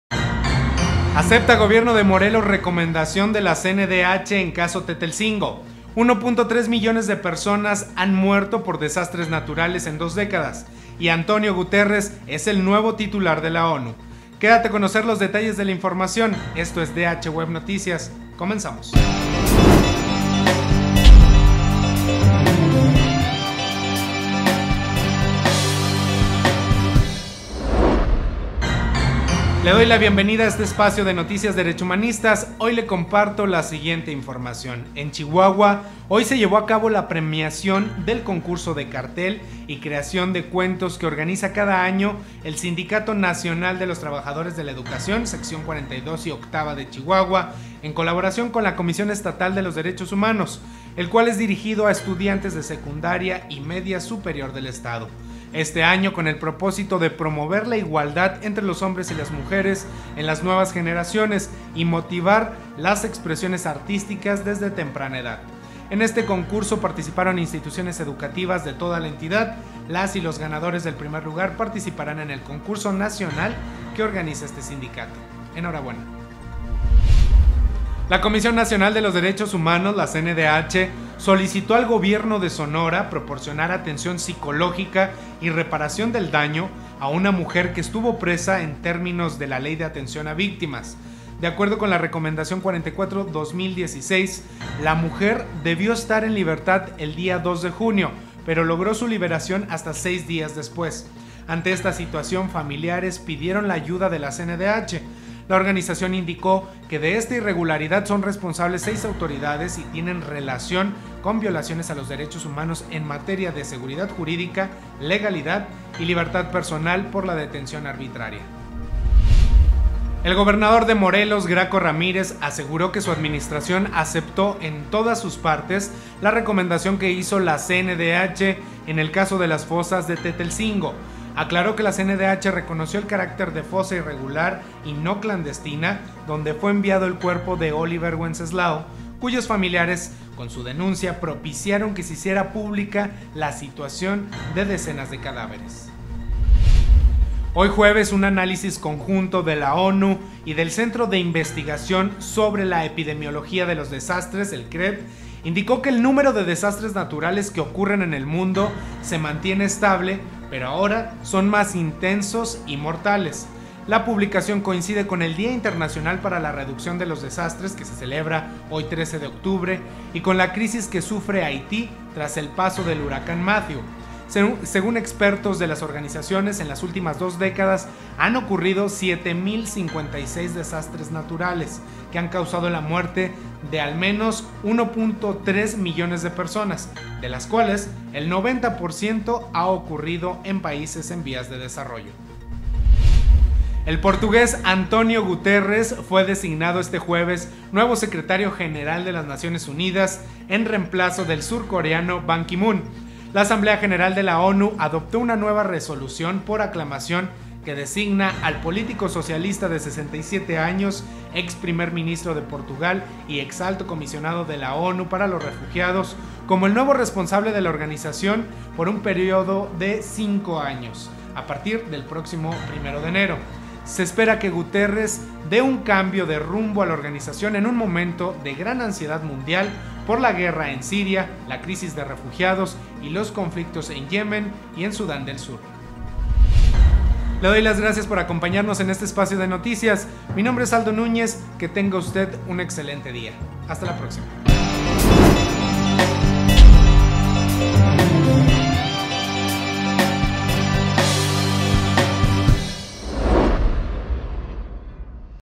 Antonio Guterres es el nuevo titular de la ONU
- Premian a jóvenes por creaciones con perspectiva de género

- Emite CNDH Recomendación al Estado de Sonora por detención arbitraria a mujer

- Acepta Gobierno de Morelos Recomendación de la CNDH en caso Tetelcingo

- 1.3 millones de personas han muerto por desastres naturales en 2 décadas